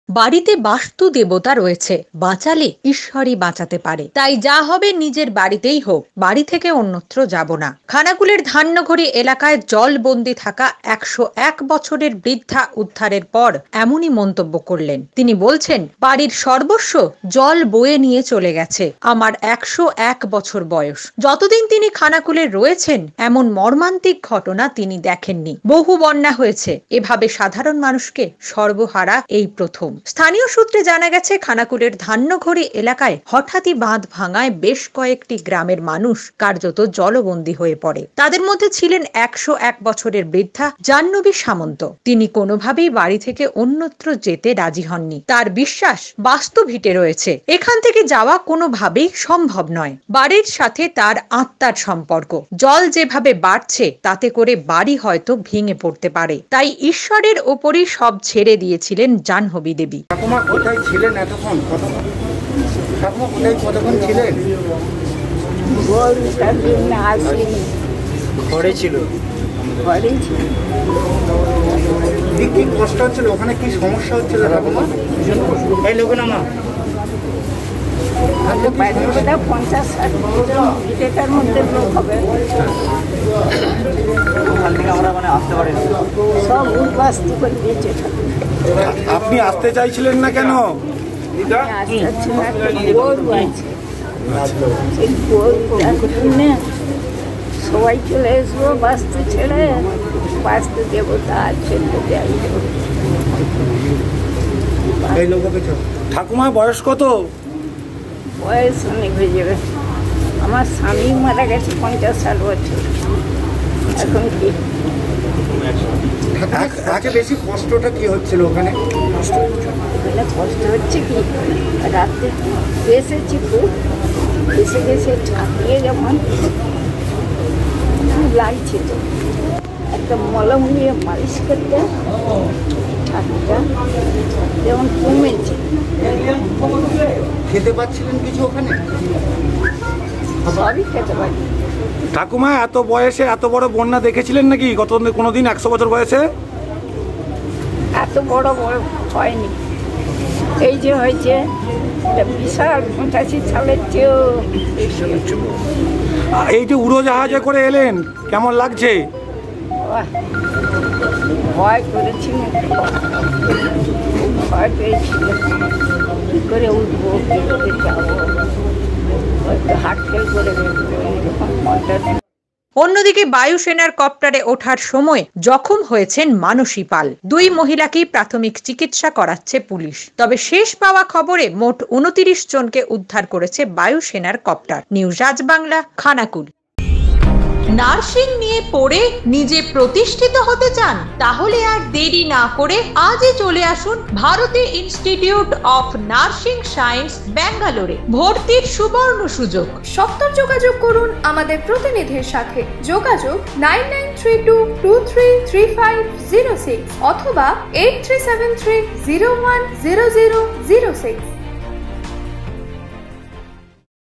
অবশেষে উদ্ধার ১০১ বছরের বন্যাদুর্গত বৃদ্ধা
বাস্তুভিটে ছেড়ে অন্যত্র যাব না, যা করার ঈশ্বরই করবেন। সদ্য জলবন্দি থেকে মুক্তি পাওয়ার পর মন্তব্য ১০১-র বৃদ্ধার।